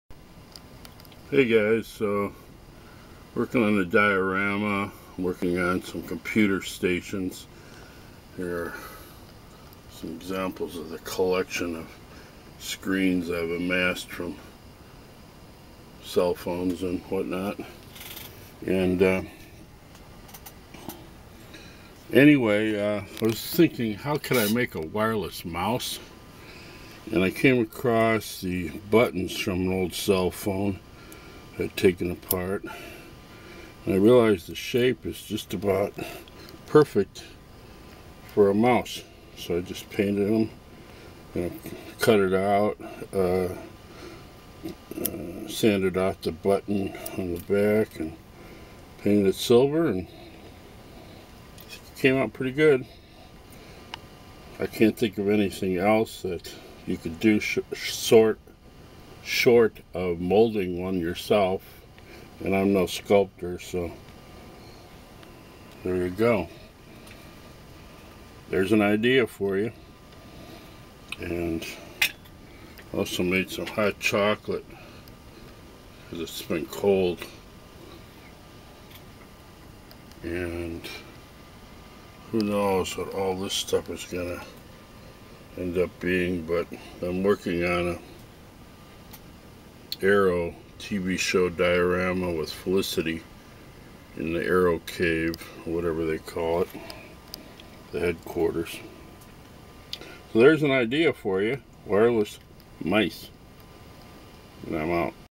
Diorama Prop Fodder: Wireless Computer Mouse
A suggestion for fodder in making a modern wireless mouse for your diorama computer set. Also a brief look at my fodder stock for computer screens and a gathering of fodder for an Arrow diorama with Felicity at her computer station . . . and hot chocolate! What more could you ask for?